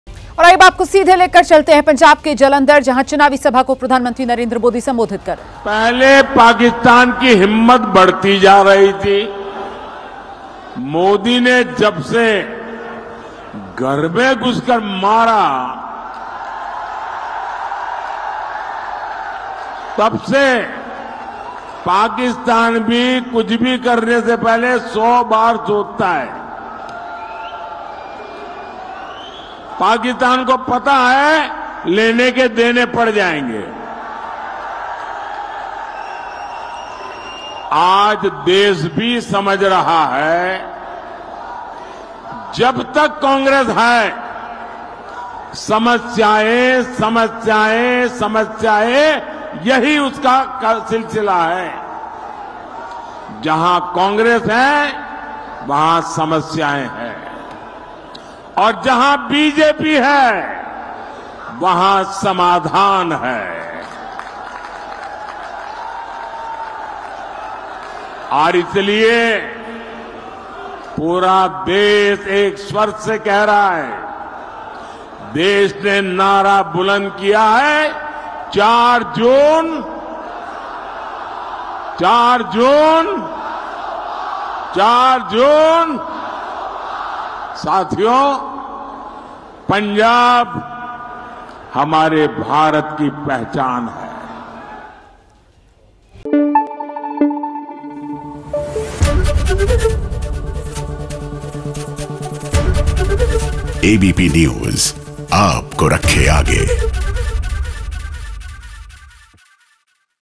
PM Modi Election Rally: जालंधर में पीएम मोदी की हुंकार..पाकिस्तान को लेकर बोले बड़ी बात | Elections
ABP News: 2024 चुनाव के दो आखिरी चरण अब भी बाकी है..और इस बीच जारी है प्रधानमंत्री की रैलियों का रेला..पीएम ने आज पंजाब के जालंधर में चुनावी रैली की और मंच से एक बार फिर पाकिस्तान पर गरजे..साथ ही विपक्षी पार्टी कांग्रेस पर भी निशाना साधते नजर आए..